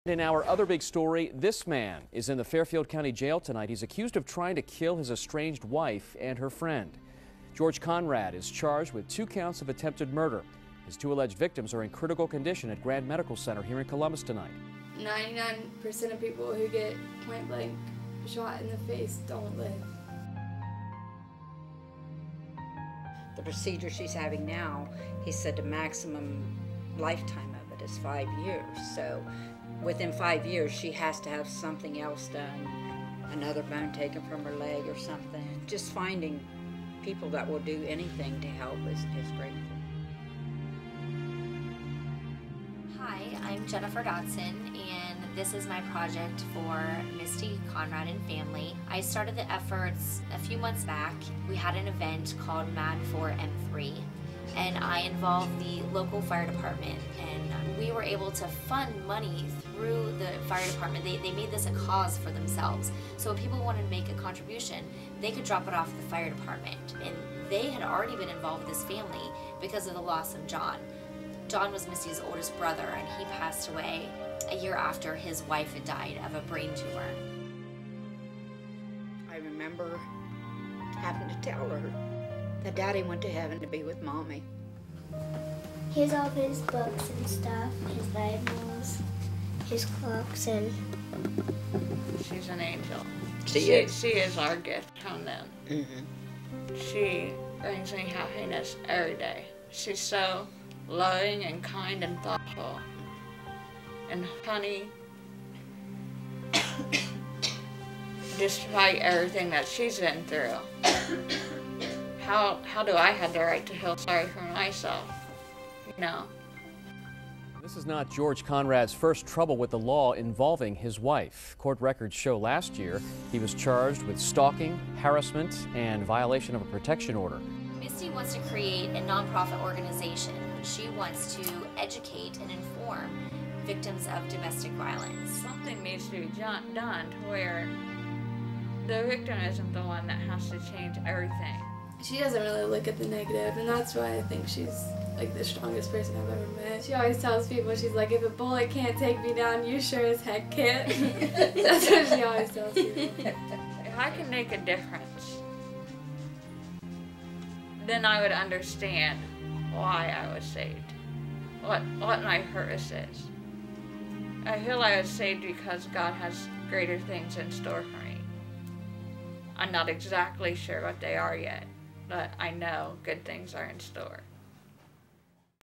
CWAM Challenge - Team Dodson - MAD 4 M3 (shorter version)
The shorter version of the charity challenge video to benefit MAD 4 M3, Misty Conrad & Family. Produced by Jennifer Dodson and Santana D. Bobo